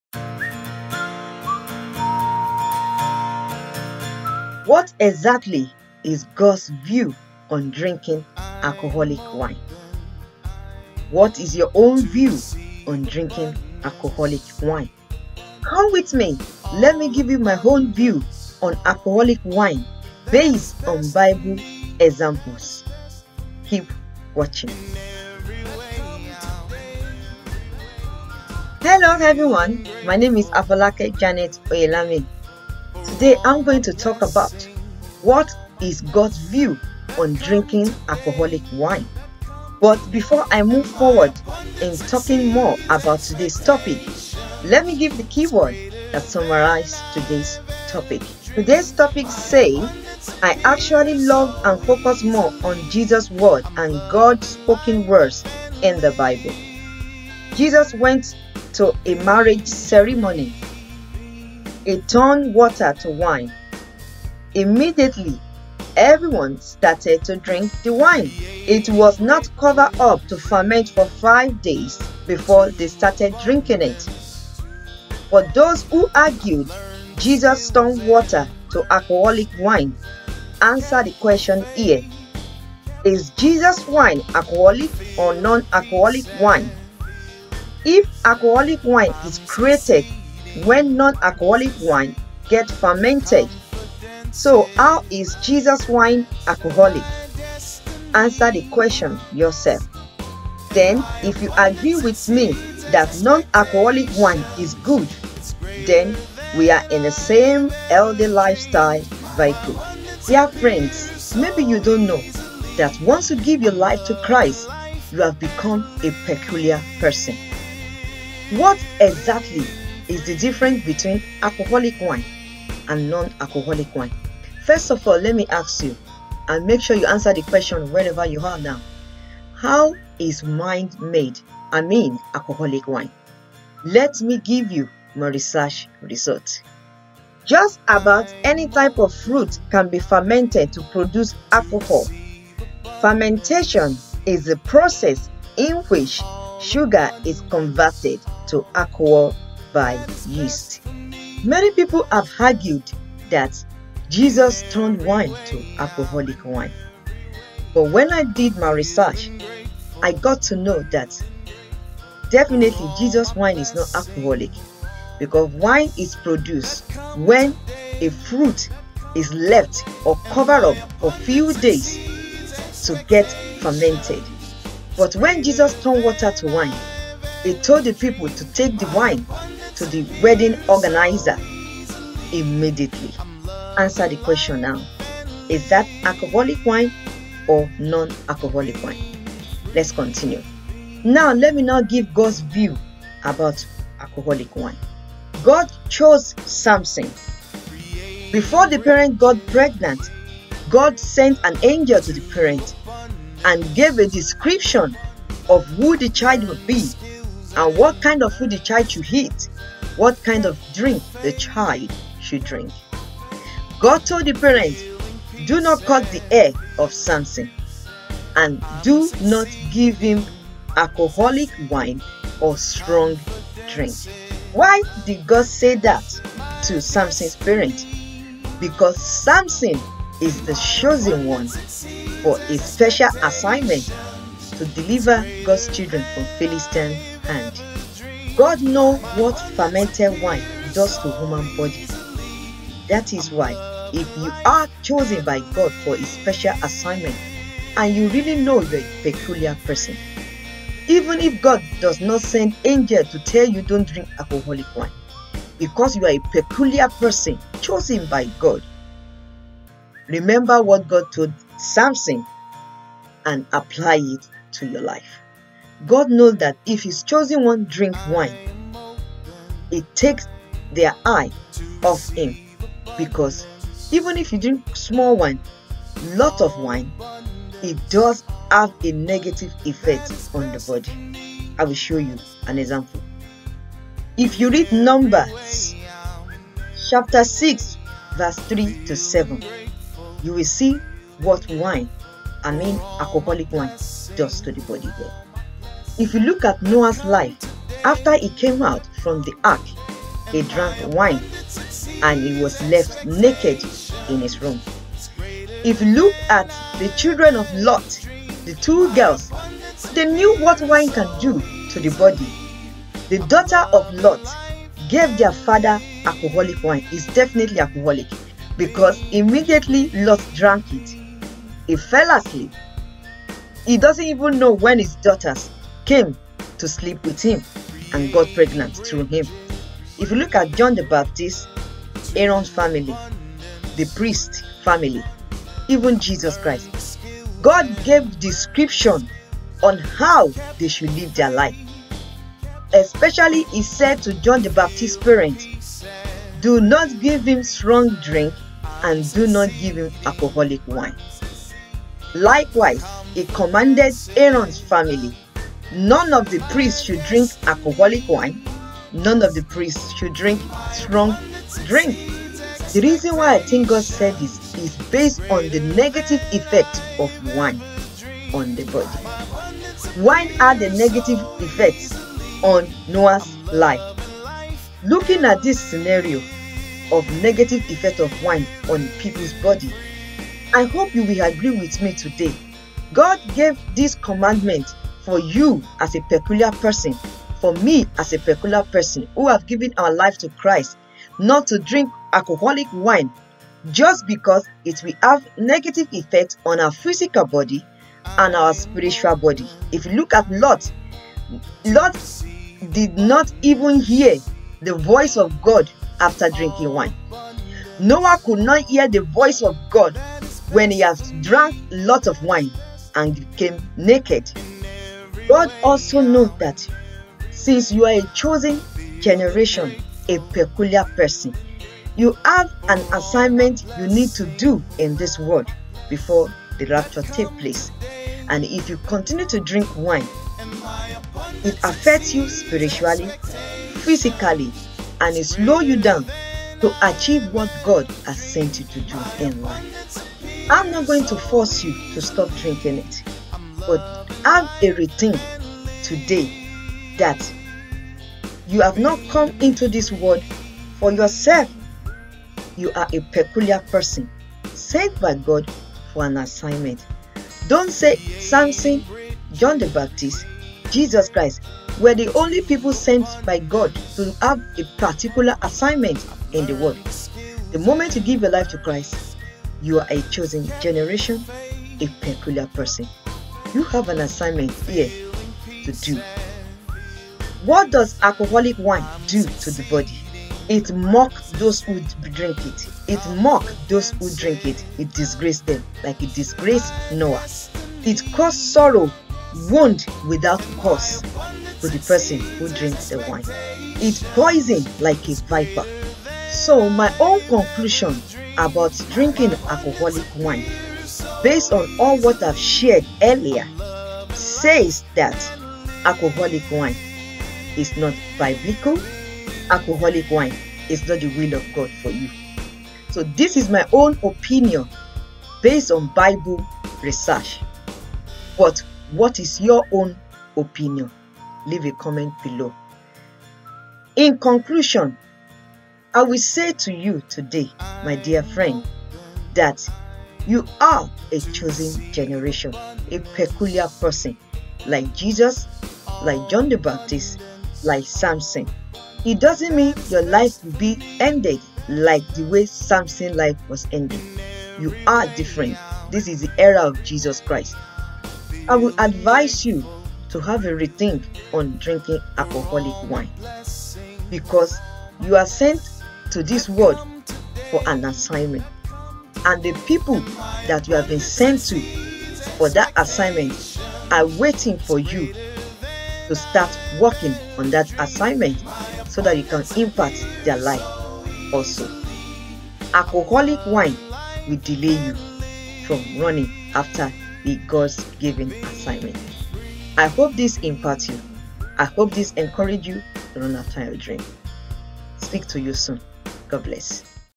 Can A Child Of God DRINK ALCOHOLIC Wine? is it a sin?
Lots of people argue if CAN A CHILD of GOD DRINK ALCOHOLIC WINE? Let's talk about it ,CAN A CHILD of GOD DRINK ALCOHOLIC WINE? Most people says Jesus turned the water to wine at the marriage  ceremony in the bible  so as A CHILD of GOD they believe they CAN DRINK ALCOHOLIC WINE since Jesus turned water to WINE.
so today let us rob mind together to know if A CHILD of GOD CAN DRINK ALCOHOLIC WINE for HEALTHY LIFE. Some CHILD of GOD even say doctor told them to DRINK WINE so  DRINKING WINE is fine to them..so kindly watch this video to the end to know exactly what Jesus and God says about ALCOHOLIC WINE.. enjoy
Seven hidden ways to discover who you are meant to be, you can't afford to miss